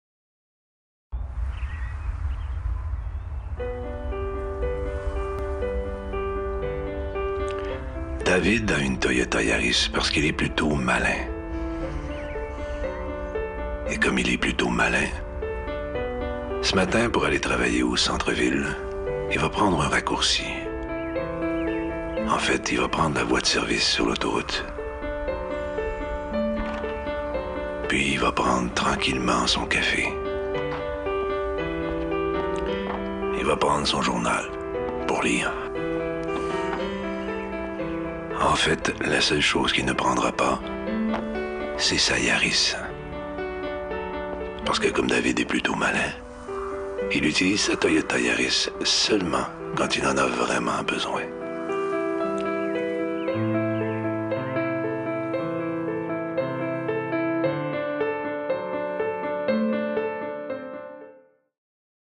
Toyota: laissez la voiture dans la cour (2/2)
Pub québécoise des véhicules toyota